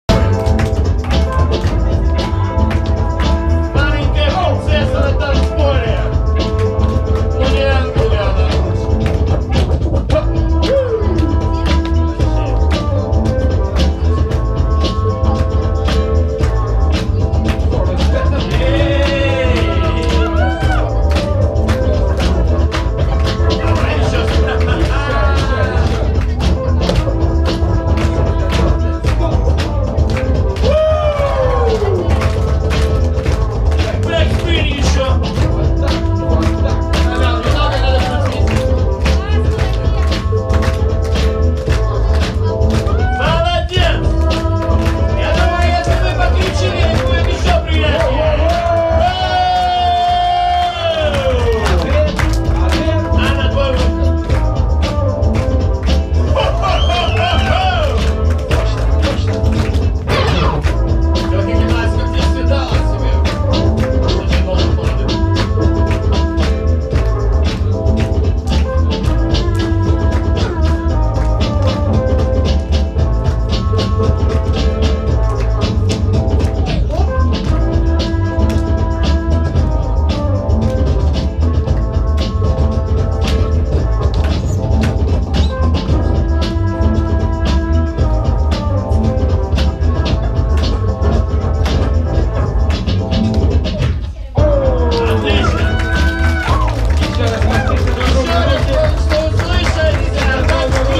Forest Battle 2019 | B-girl Аня vs B-girl Ангелина - Tie
Forest Battle
B-girls (до 2х лет опытом)
B-girl Аня vs B-girl Ангелина
B-girl Аня - won
Belarus, Borovlyany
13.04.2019